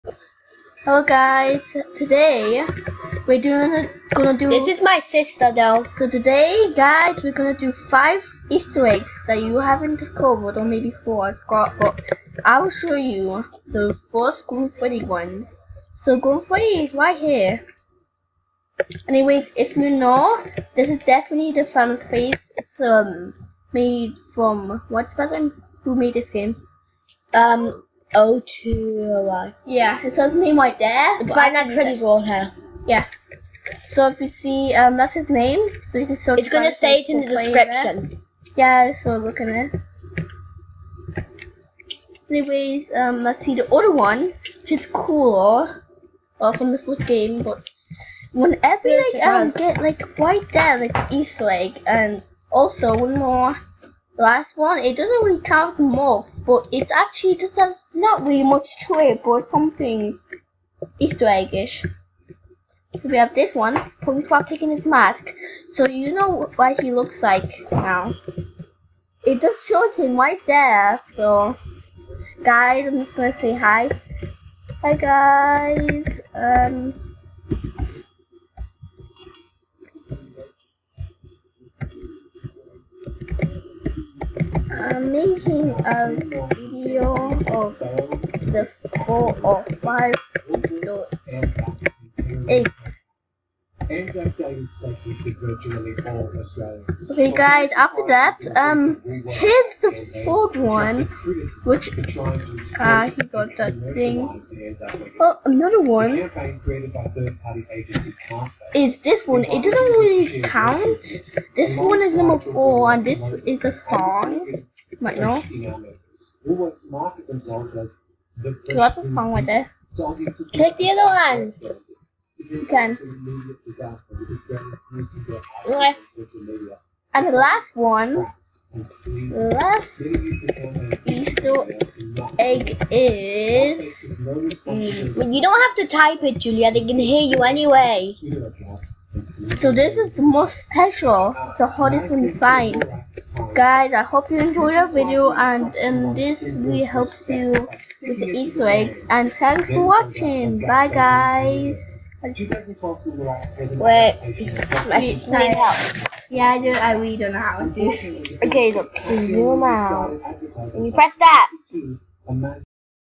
Easter Eggs (By My Sister)
Sorry she has a speech inpedement